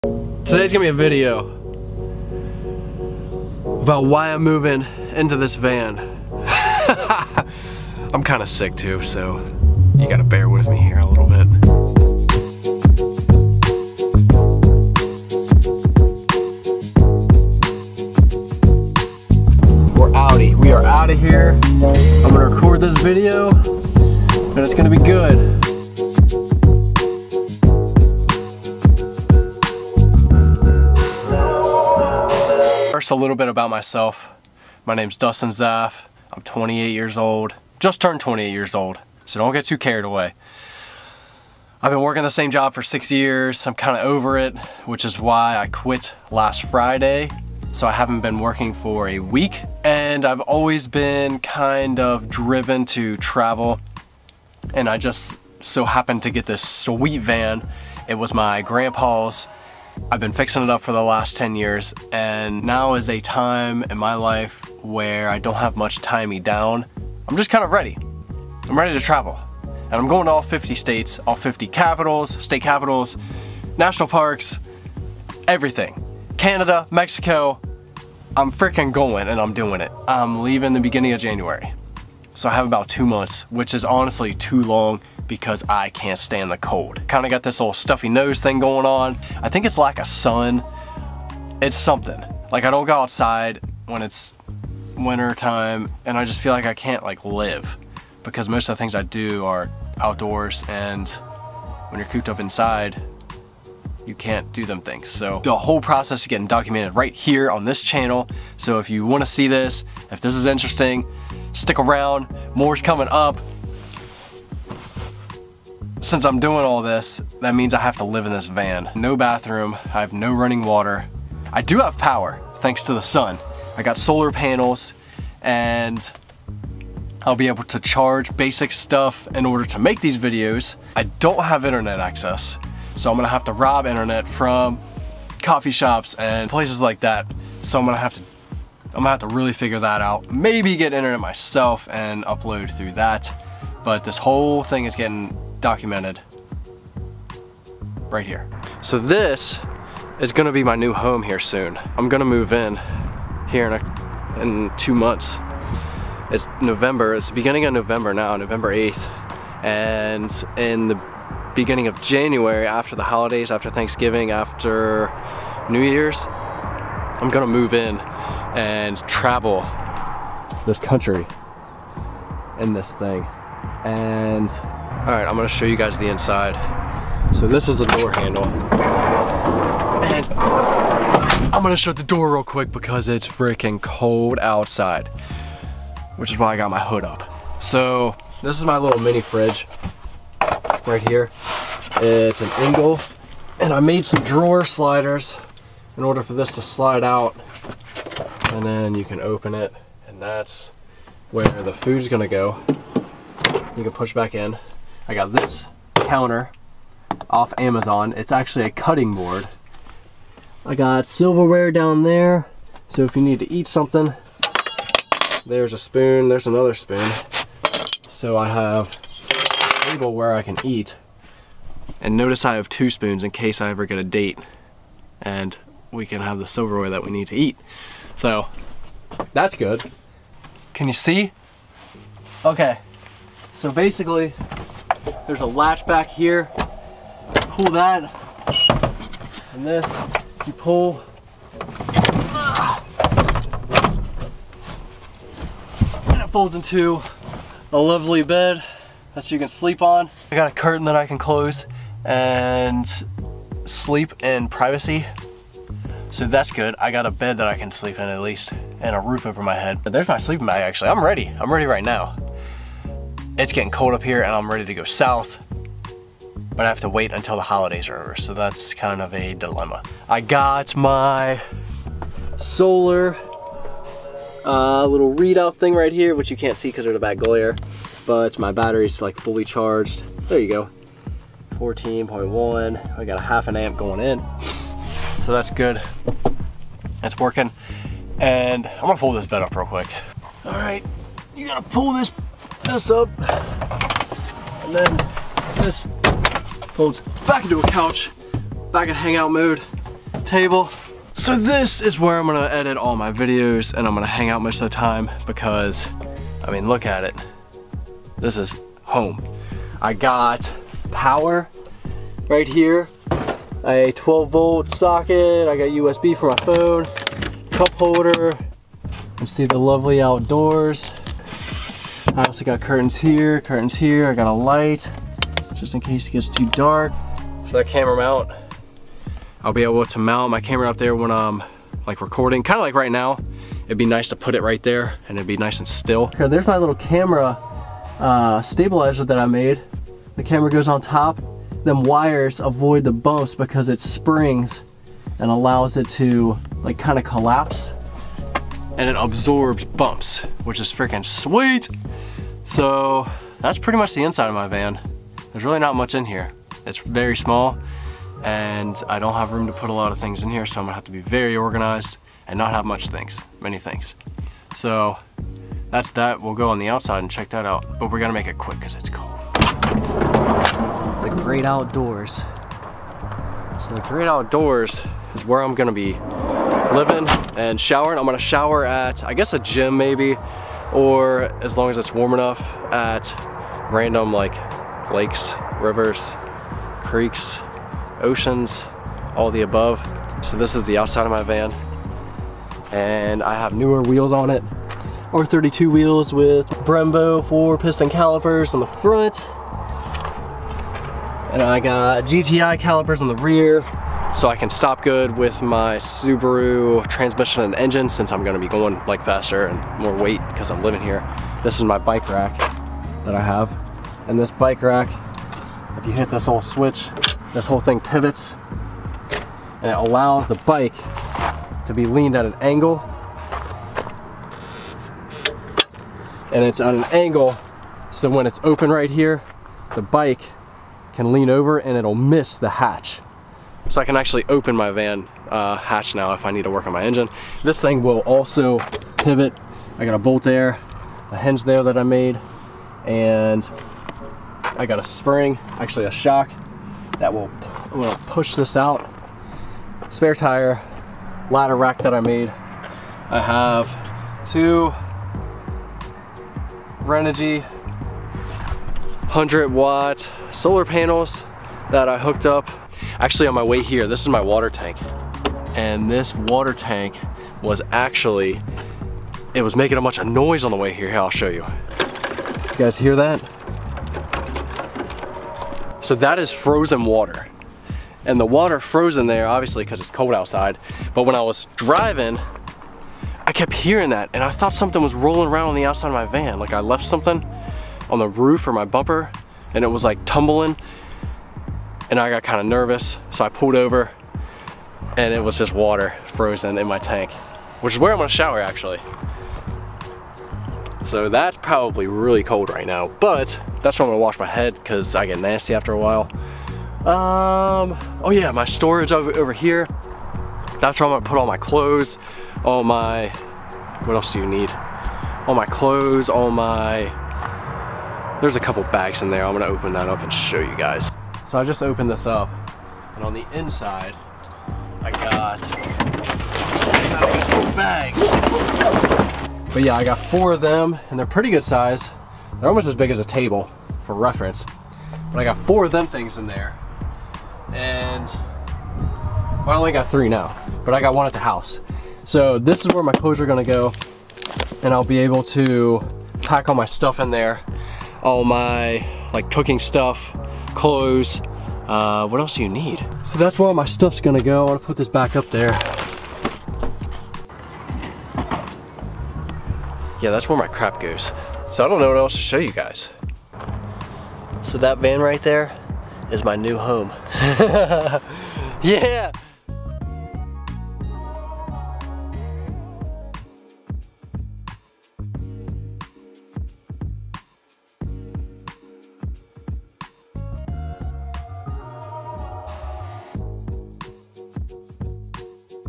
Moving into a van
This is a little video I made about the van in going to live in while driving across all 50 US States!! I built this van to the best of my ability and made it as convenient to live in as possible. Subaru engine, Subaru transmission, newer brakes, wheels, suspension, EVERYTHING!!! Lets hope it makes it. Stick around, will be uploading daily!!!